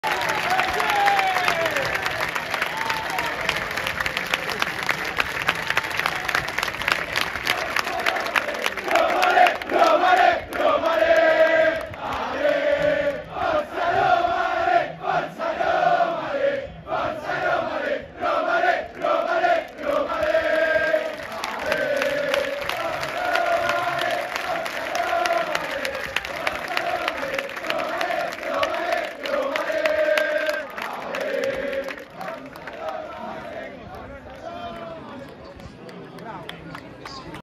27/5/23 Fiorentina vs AS Roma 2-1 ... entra in campo la ''primavera'' della AS Roma ...
giornata estiva in quel di Firenze per l'ultima trasferta stagionale di Serie A ... i giocatori giallorossi, in gran parte della Primavera , scendono in campo per effettuare il riscaldamento prepartita ... e anche oggi il settore ospiti è ancora esaurito nonostante che questa partita non conti assolutamente nulla